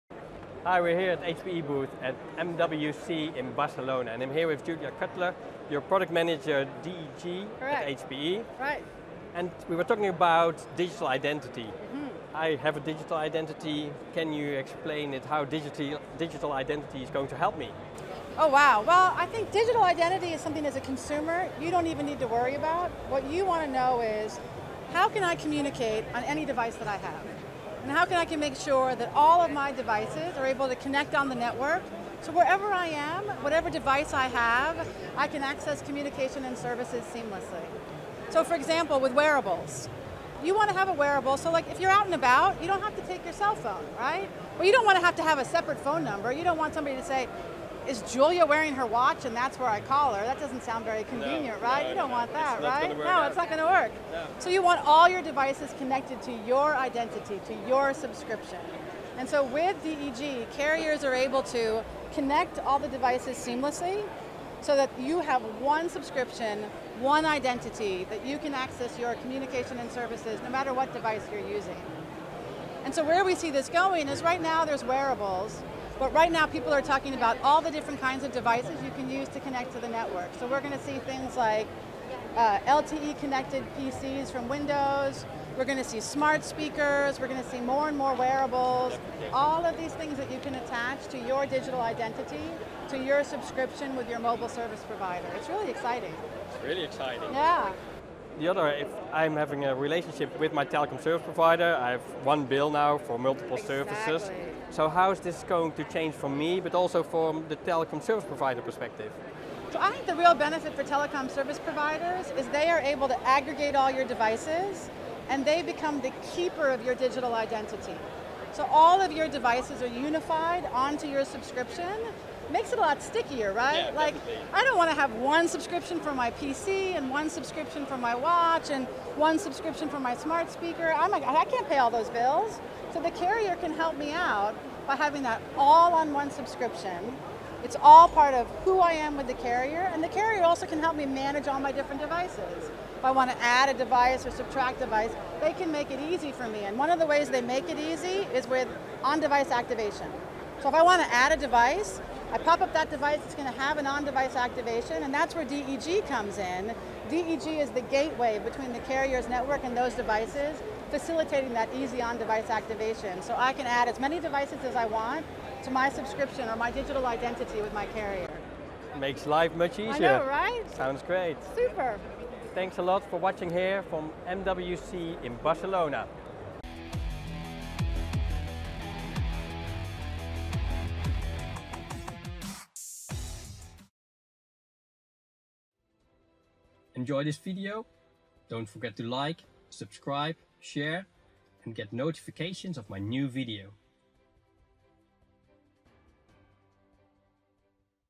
One Digital Identity For All Your Connected Devices
A live interview with Julia Cutler, Product Manager, Communications & Media Solutions at HPE, about One Digital Identity for all your Connected Devices.